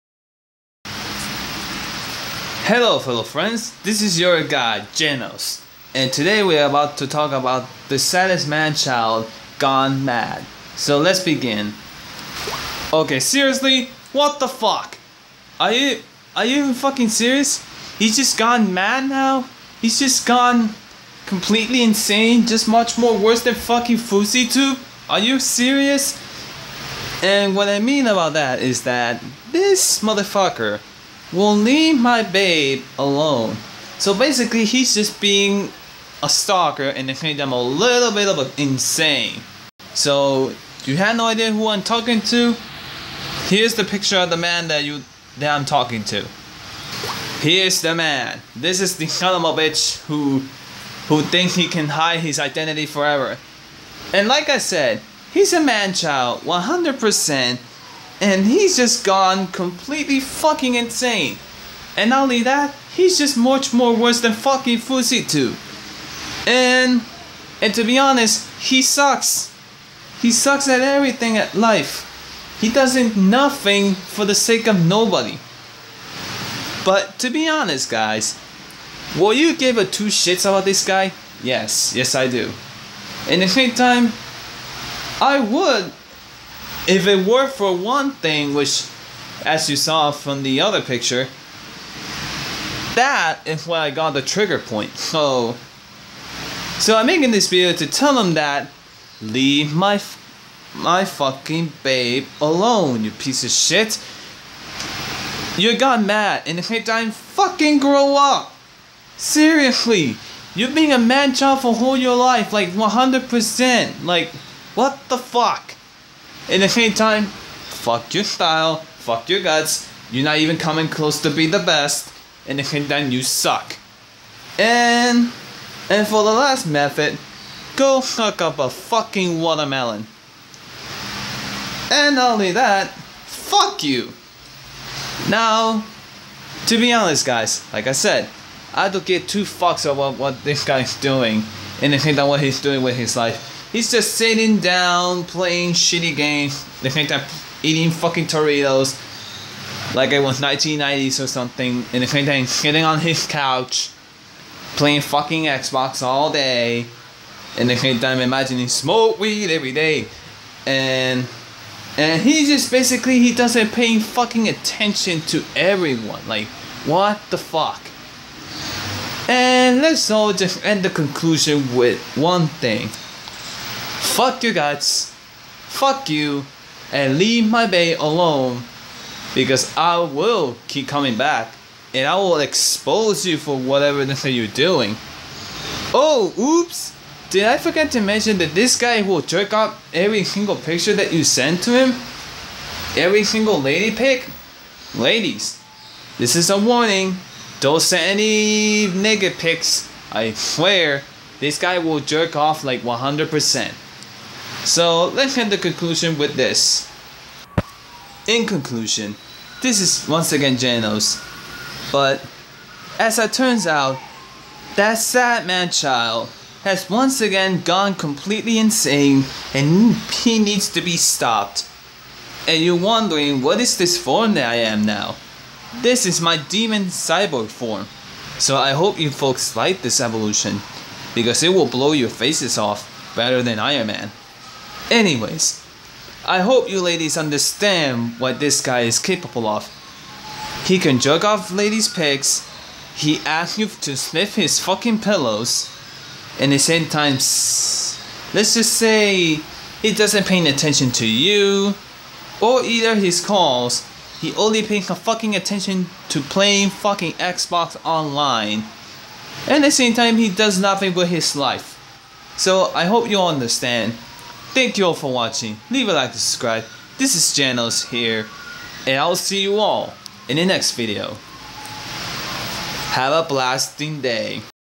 😑the manchild has go mad like WTF😑
Hi guys please can someone tell him that he needs to grow up like seriously😑and don't listen to him okay ladys he never pays attention to you😑(I hope you guys like my demon cyborg from✌😉)

This is Genos here

I'll see you in next video. have a blasting day✌😉